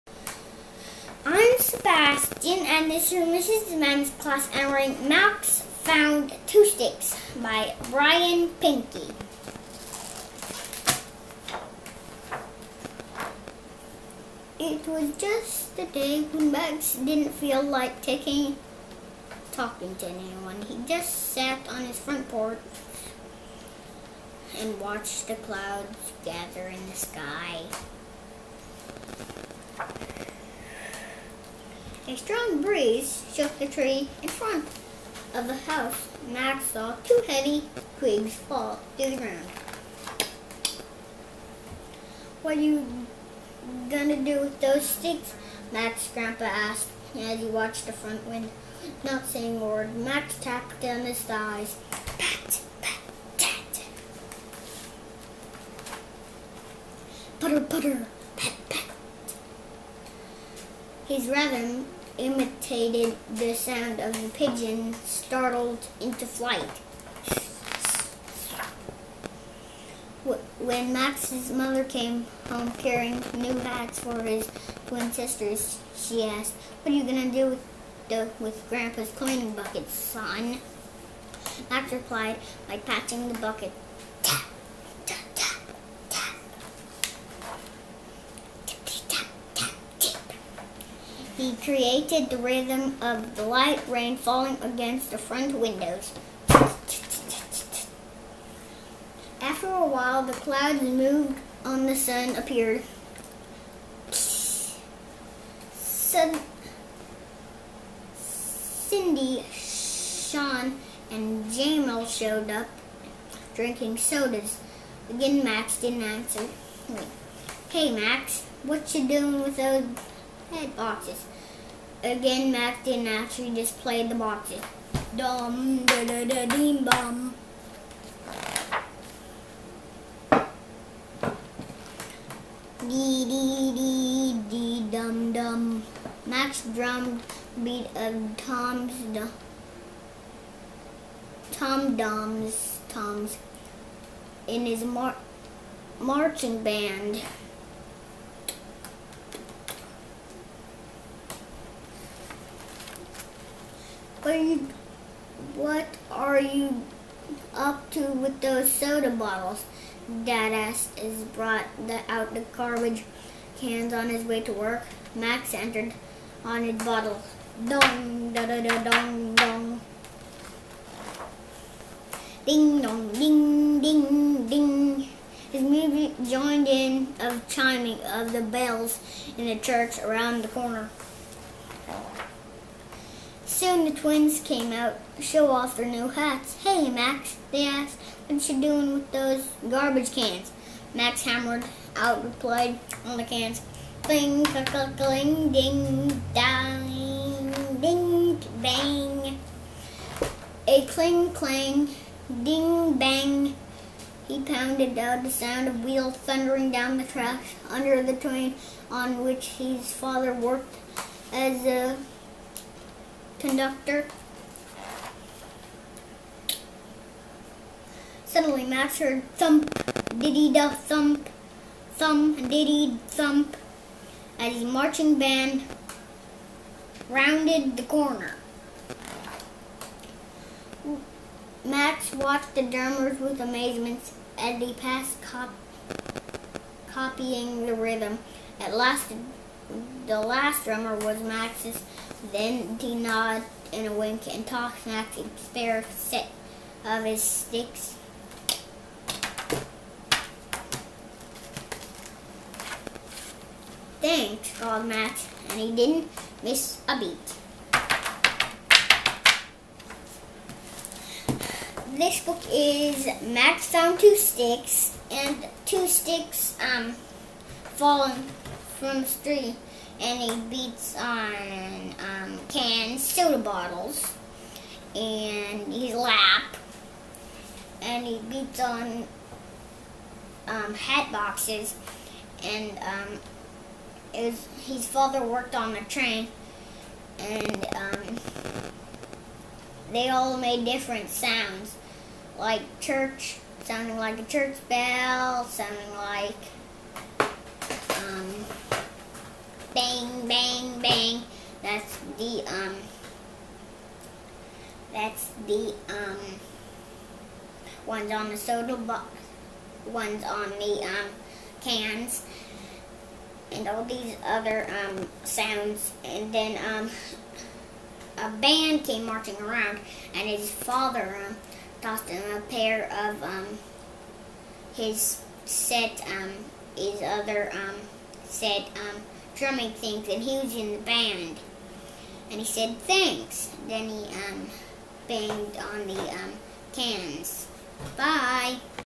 Max Found Two Sticks Being Read by Sebastian Hamilton for Mrs. Mann's Class
Sebastian reading Max Found Two Sticks by Brian Pinkney for Mrs. Mann's Class on March 16, 2014.